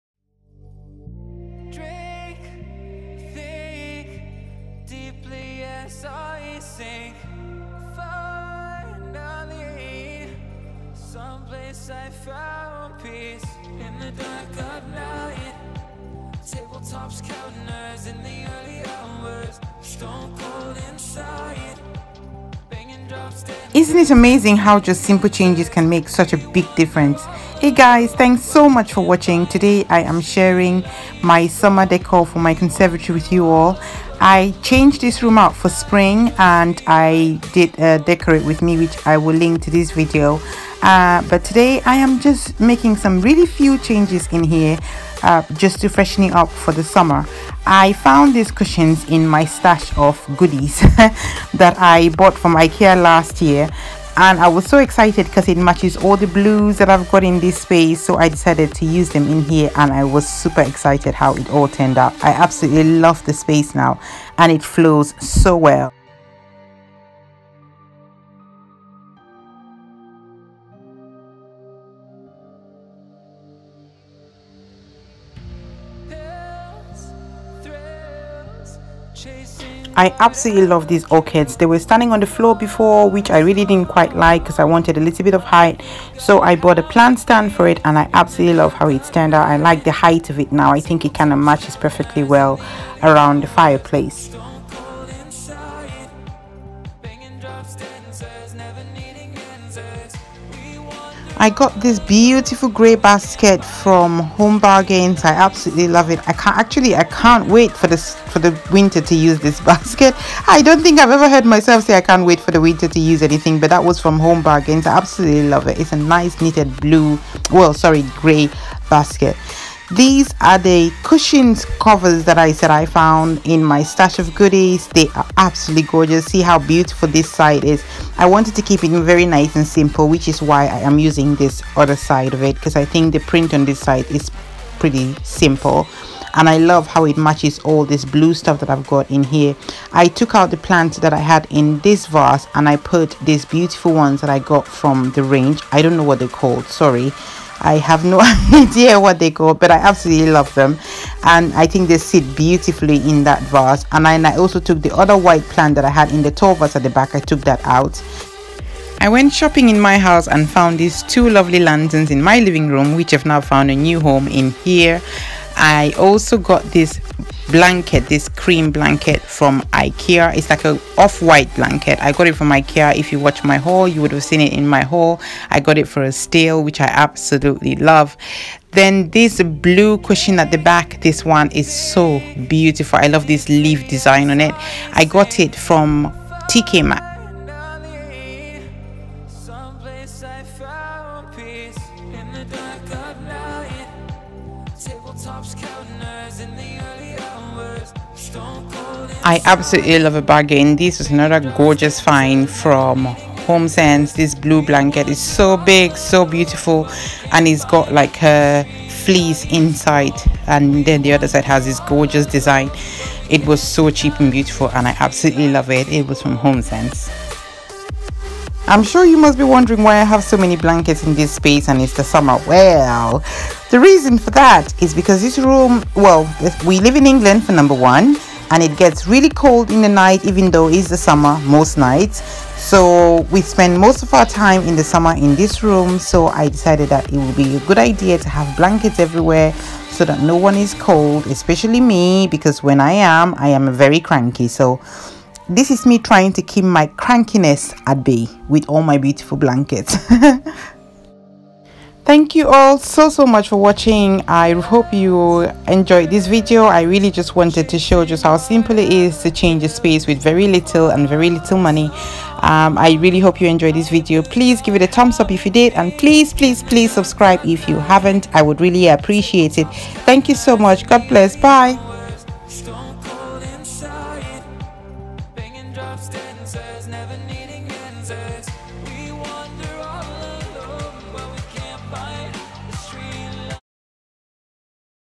Family Room Summer Decor Tour
this my favourite room in my home in the summer so I always decorate it first!! i wanted blue this summer and that was inspired by the blue and white vases I had in this space. I love this look and I am looking forward to enjoying his space.